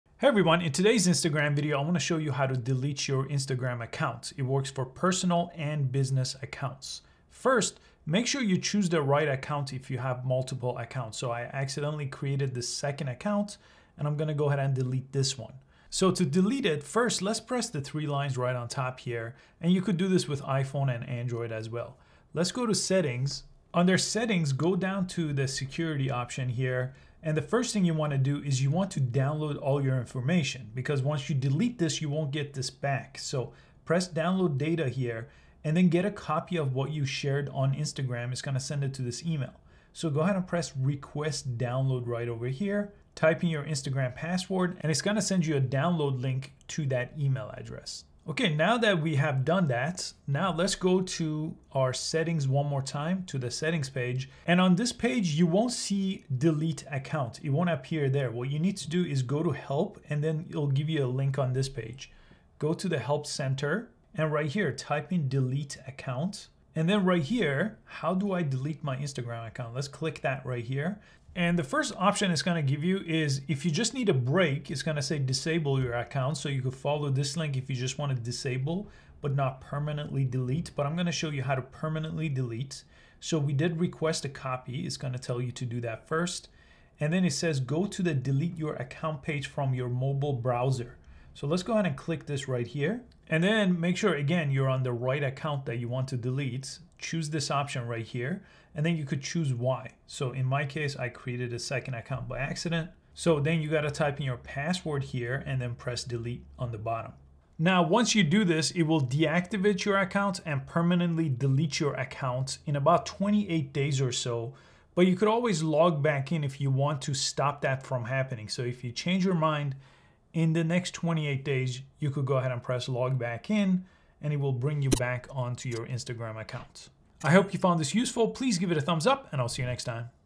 How to Delete Your Instagram Account
Need to delete Your Instagram account? Instagram allows you to temporarily disable your account or permanently delete your account.

To delete your Instagram account, make sure to download a copy of your Instagram data first.  Because once you delete your account, you won’t be able to get this back.

Once you have that, you can delete your account by using the Instagram help menu in the Instagram settings.  Simply type in delete the account and Instagram will provide a link for you.

Choose a reason why you are deleting Instagram, type in your password, and press delete account.